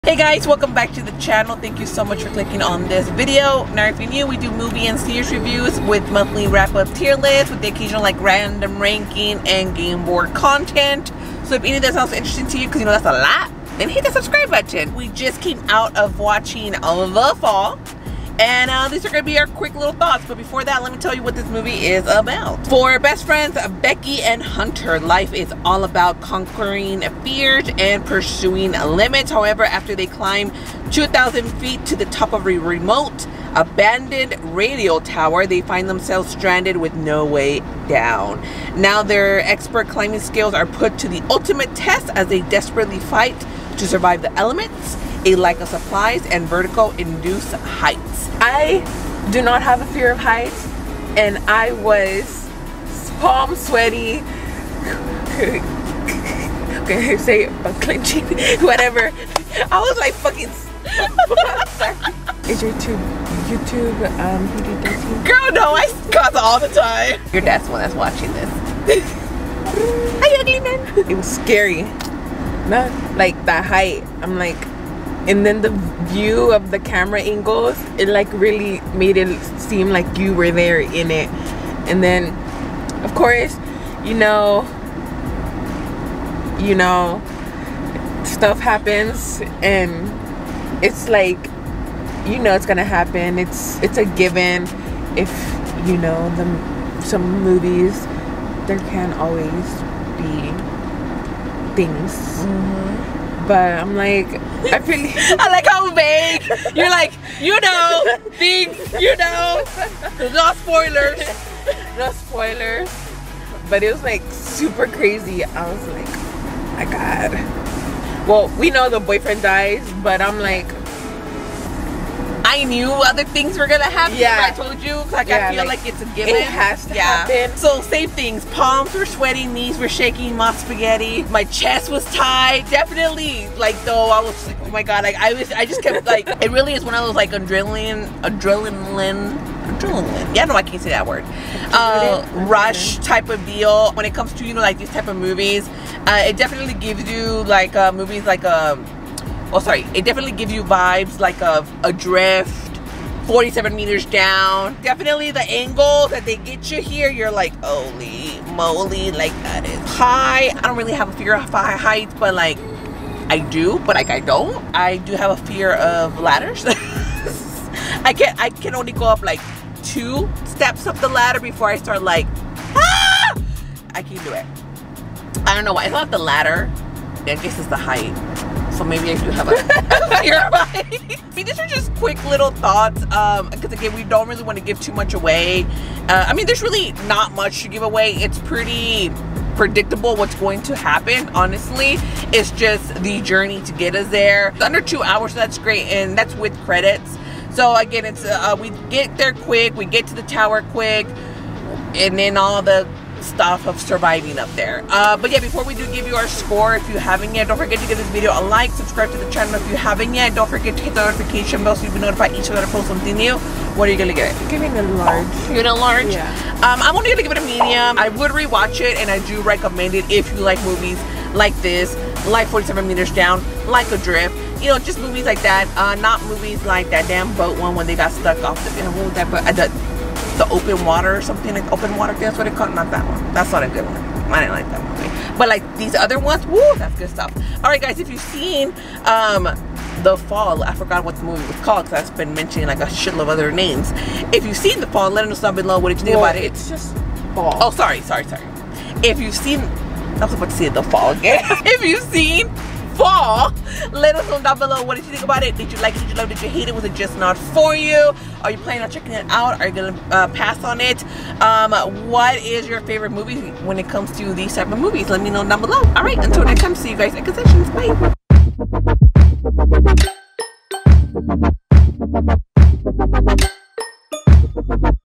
Just Watched FALL ⎮ Our Thoughts
I'll Buy the Popcorn's host Stephanie and her sister Desarae just saw FALL and these are their thoughts.

Rating System can be taken as follows:
Large Popcorn 👉🏼LOVE IT!
Medium Popcorn 👉🏼Like it
Small Popcorn 👉🏼It's Okay
Popcorn Kernal (Water Cup)👉🏼Hate it